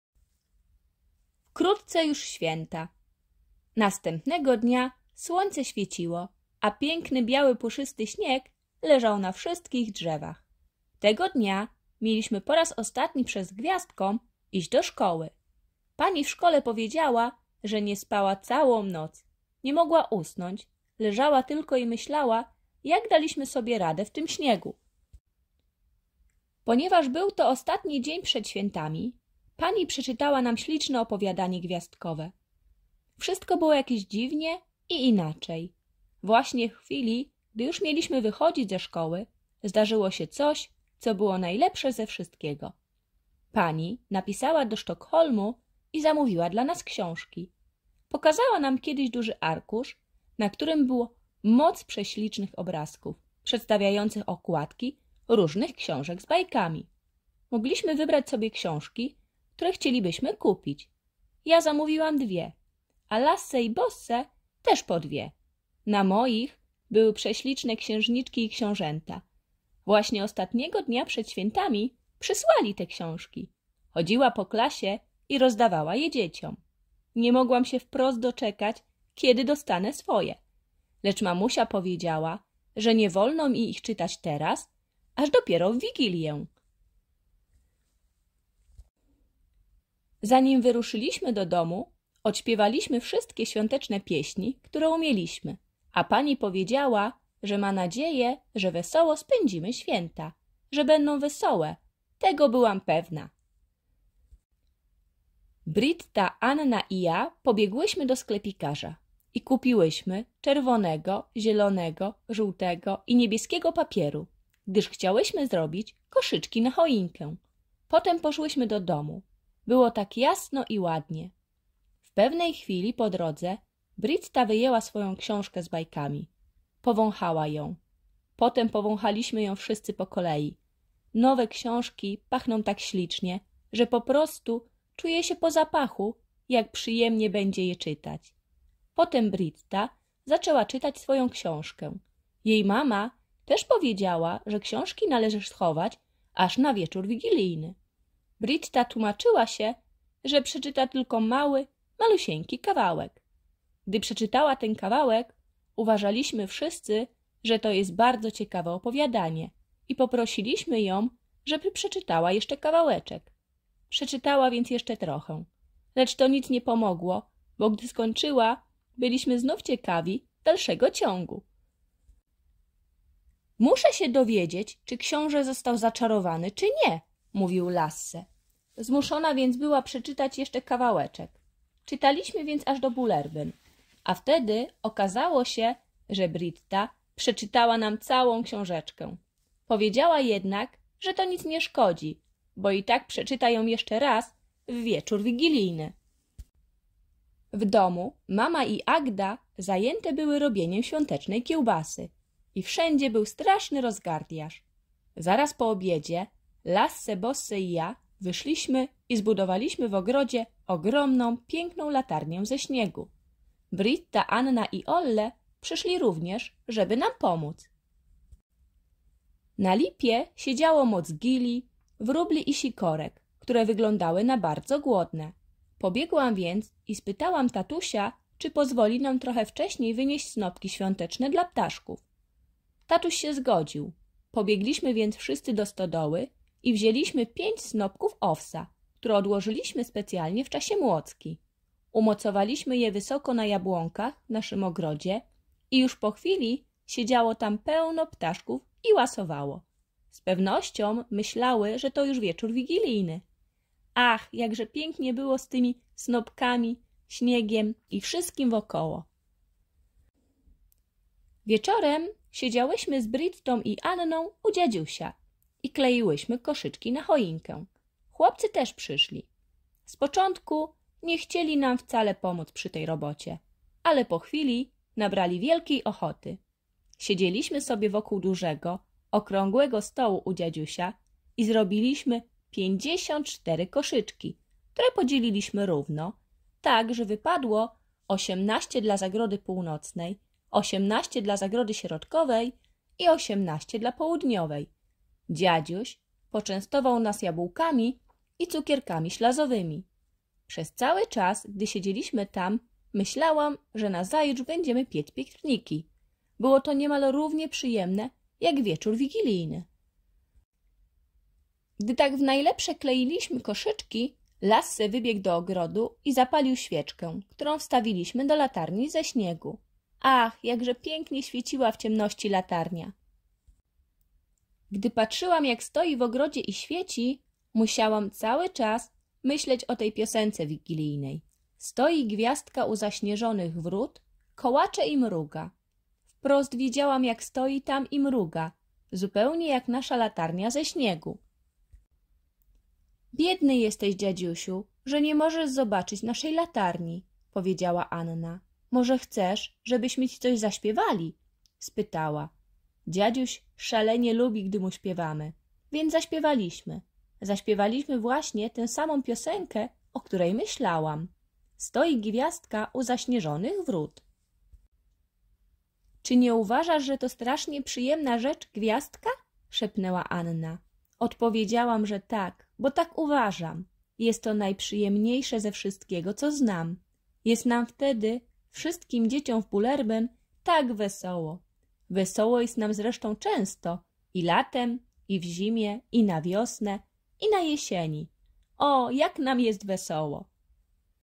🏘 DZIECI Z BULLERBYN Astrid Lindgren Audiobook 17 🏘
Astrid Lindgren

DZIECI Z BULLERBYN

CZEŚĆ 1

WKRÓTCE JUŻ ŚWIĘTA!

Lasse, Bosse, Lisa, Olle, Britta i Anna mieszkają w Bullerbyn. To maleńka wioska, ale wcale nie jest tam nudno. Każdy dzień, z pozoru zwyczajny, staje się świetną przygodą. Można łowić raki, spać w stogu siana, szukać skarbów, założyć spółdzielnie wiśniową oraz przesyłać wiadomości sznurkową pocztą. A najważniejsze, że przyjaciele i rodzina zawsze są blisko.

Opowieść o dzieciach z Bullerbyn to ukochana lektura z dzieciństwa kilku pokoleń czytelników.